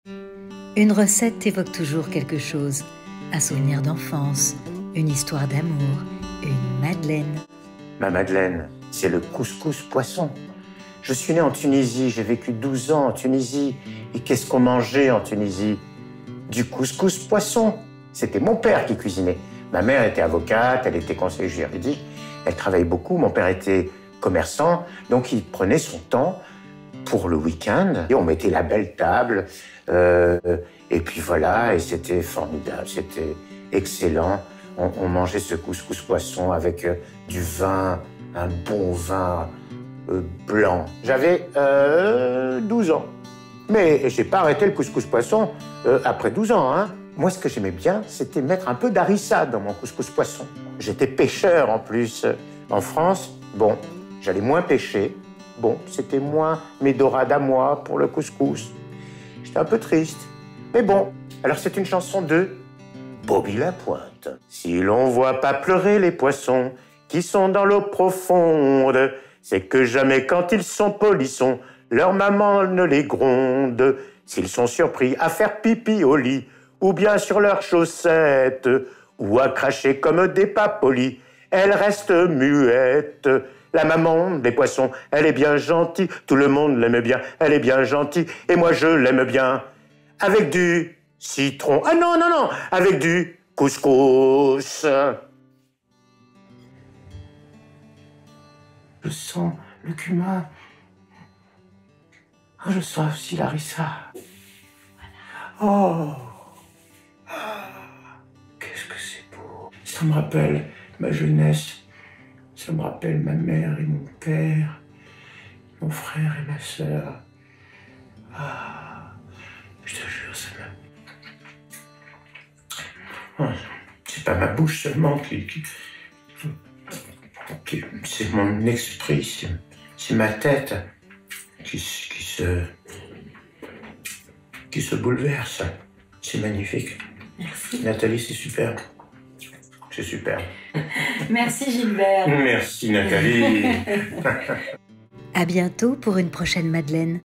La Madeleine de Gilbert Levy
Nous avons tous une recette qui est notre Madeleine. Gilbert Levy vous parle de la sienne (le couscous poisson), des souvenirs liés à cette recette.

Gilbert Levy est un comédien spécialisé dans le doublage, vous l'avez surement entendu dans South Park ou Les Simpson.